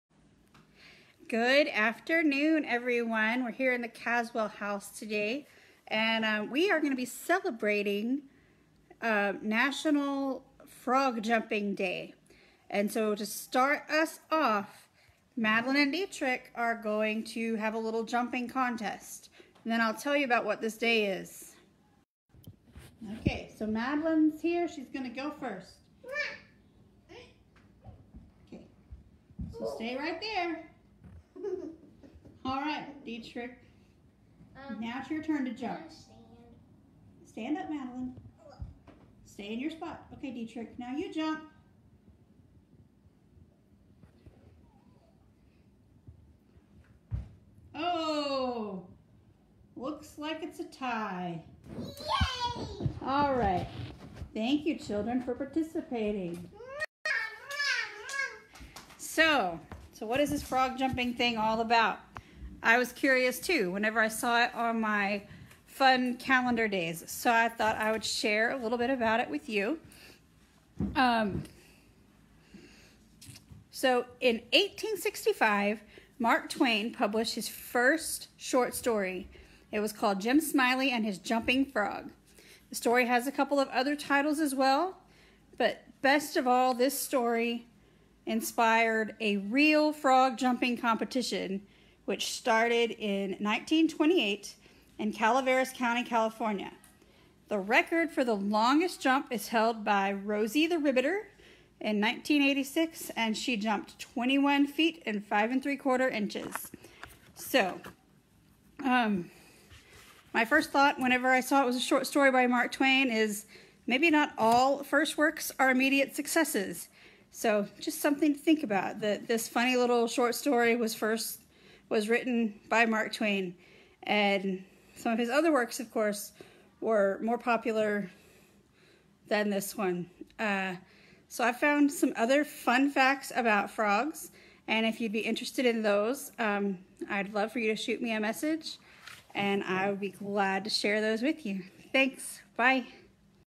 How to Leap over Obstacles
May 13 is Frog Jumping Day. Learn about this funny day and how you can hop over your obstacles.